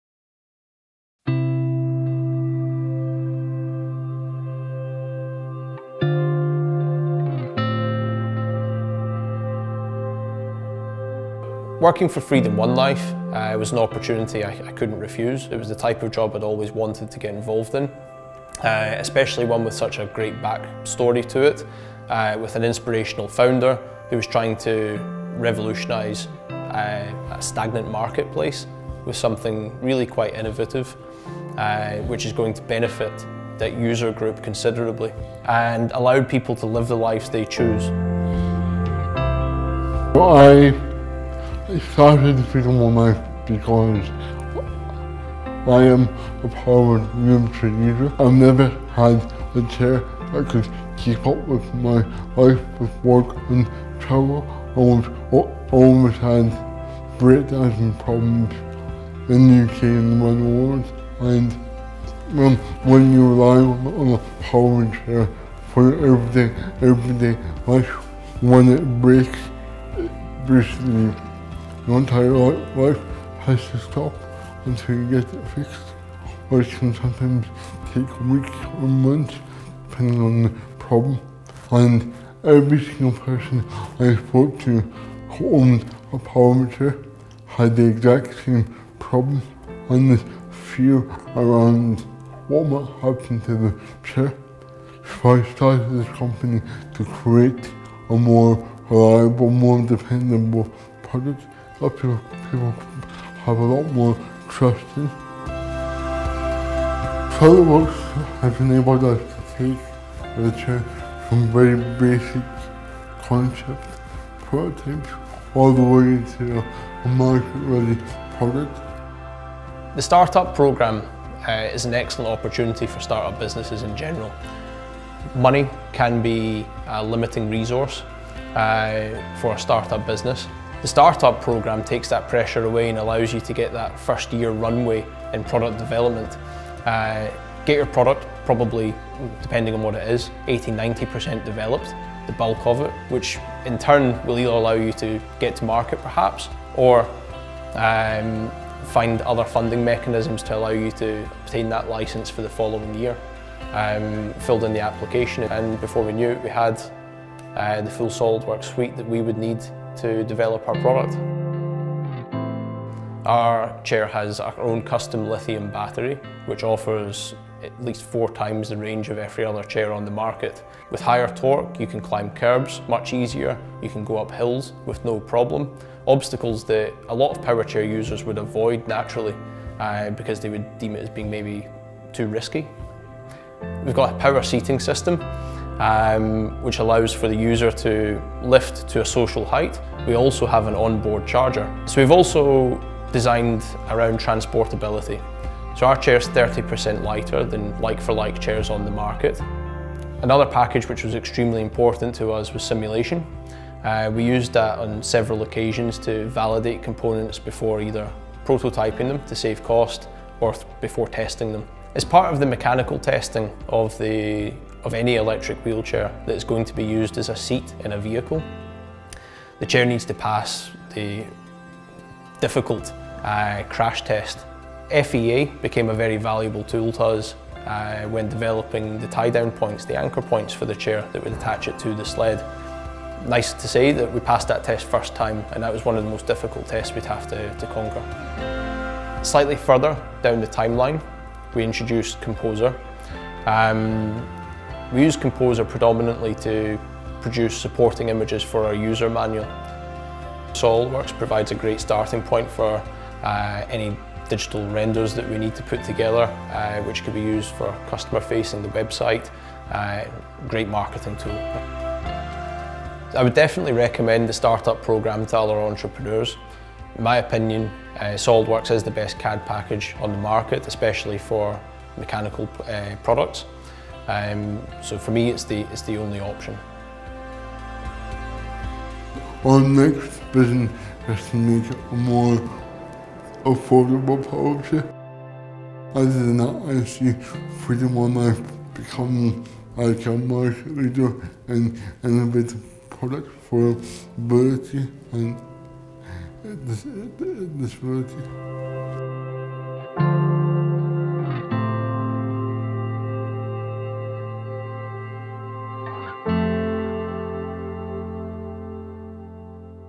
Freedom One Life & the 3DEXPERIENCE Works for Start-ups Program change the powered wheelchair market
Freedom One life was founded by a powered wheelchair user, Alex Papanikolaou, who didn't want to be limited by the current powered wheelchair market.

In 2019, Freedom One Life joined the SOLIDWORKS Startups Program. Freedom One Life are disrupting the powered wheelchair market with the Series 5. The journey designing the Series 5 required the use of SOLIDWORKS to create concepts, validate designs and finally design parts and assemblies for manufacture. By the end of 2019 they had a working prototype. Now with medical device testing underway, Freedom One Life are on track to manufacture and supply the Freedom One Life Series 5.

"I would absolutely recommend SOLIDWORKS Startups Program to other entrepreneurs. It’s great the support you can get from both SOLIDWORKS and TMS CADCentre and it’s a really good way to invest in start-ups. The first year is always difficult and to have access to SOLIDWORKS makes a real difference. I’ve told everyone I know and work with about the SOLIDWORKS Startups Program and how good the benefits are.”
Scott Savage, Technical Director.